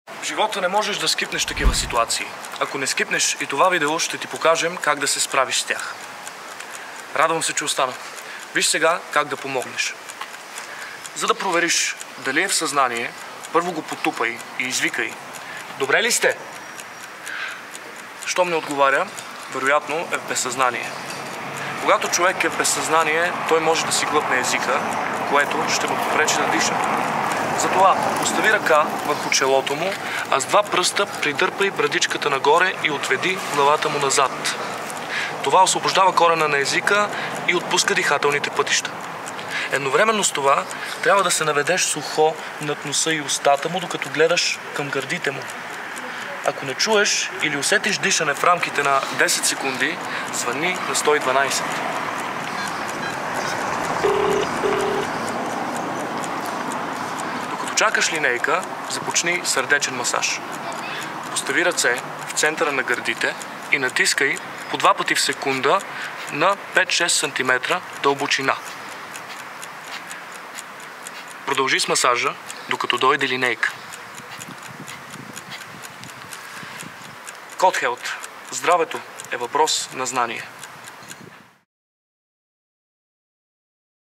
Как да помогнеш при гълтане на език?
Понякога не можем да избегнем неприятните ситуации в живота.
Понякога някой има нужда от нашата помощ.
Може да е близък, може да е непознат.
Когато знаеш какво да направиш с човек, който е глътнал езика си, можеш да спасиш живота му.

Ние от Code Health, създадохме това видео съвместно Теодора Георгиева, консултант от фондация “Първите три минути”, за да знаеш да реагираш правилно в подобна ситуация, защото здравето е въпрос на знание.

Търсете Cоde Health TV при Вашия оператор, както и 24/7 на

Здравето е въпрос на знание!